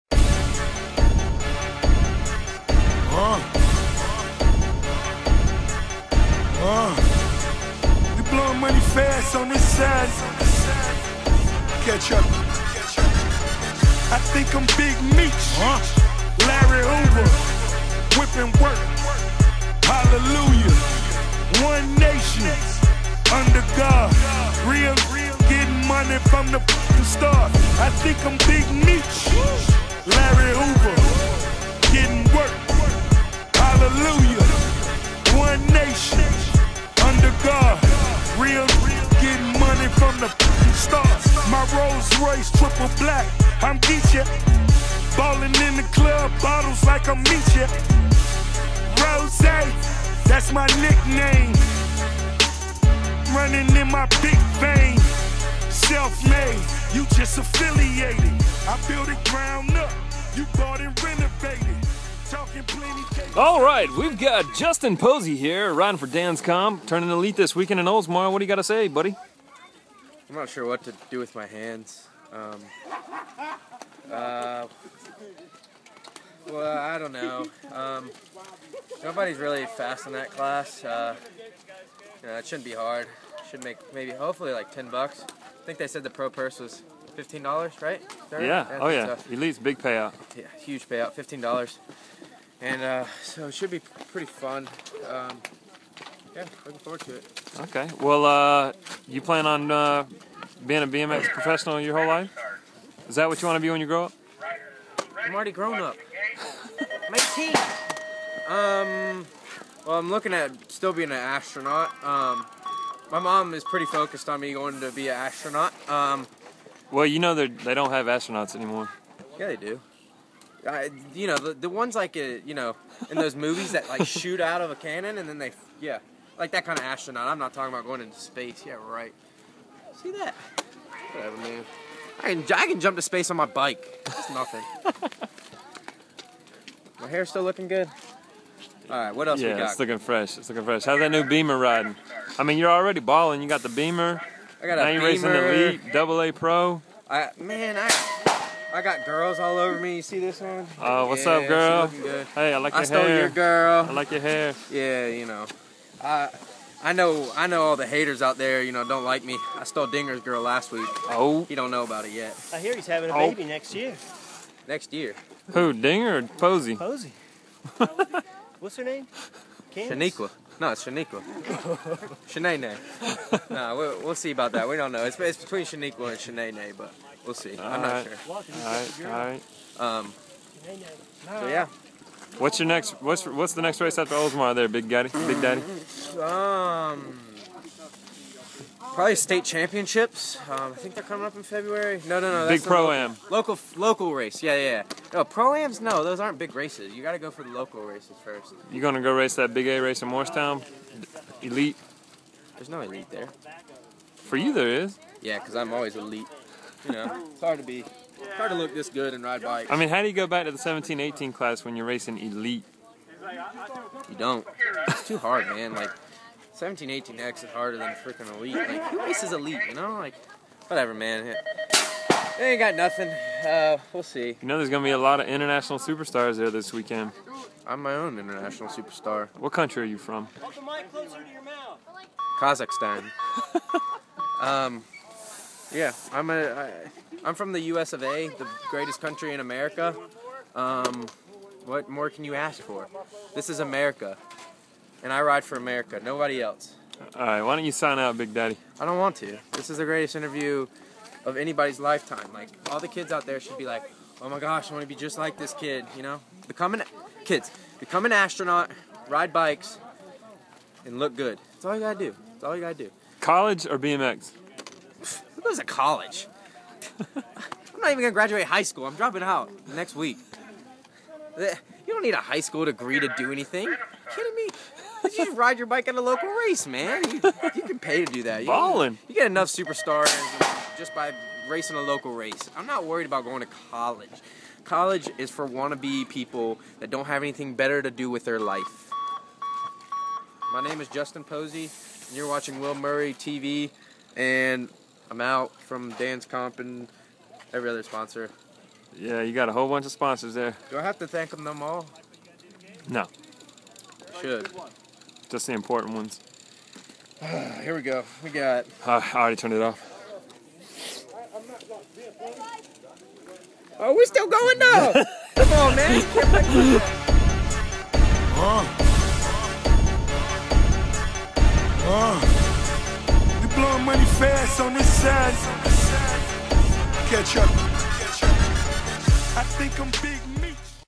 Justin Posey part duex
Justin Posey is ready for Elite and to be an astronaut. Blastin off at a track near you!